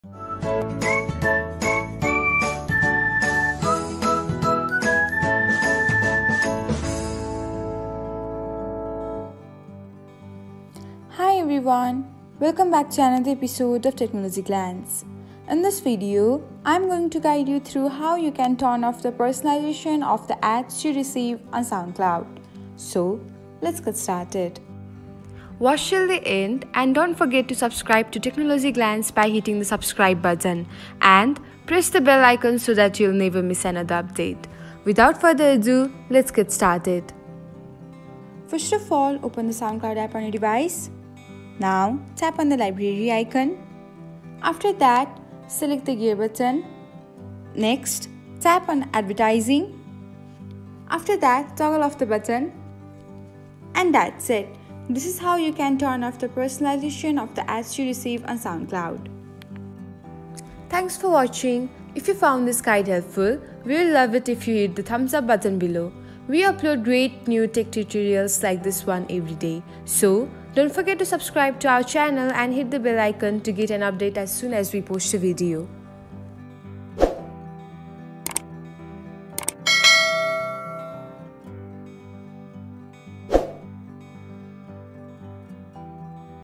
Stop Soundcloud Ads Personalization - How to Disable it | Technologyglance
Do you want to turn off ads personalization on Soundcloud and protect your privacy? Soundcloud collects data on your listening habits to personalize ads that are shown to you. While this feature is designed to enhance your experience, it can be concerning for those who value their privacy.

In this tutorial, we will guide you through the process of turning off ads personalization on Soundcloud. By disabling this feature, Soundcloud will no longer use your listening habits to personalize the ads that you see.

We'll start by showing you how to access the settings menu on Soundcloud. Then, we'll guide you through the process of turning off ads personalization.

By the end of this tutorial, you'll be able to disable ads personalization on Soundcloud and enjoy a more private listening experience. Follow our simple guide and turn off ads personalization on Soundcloud today.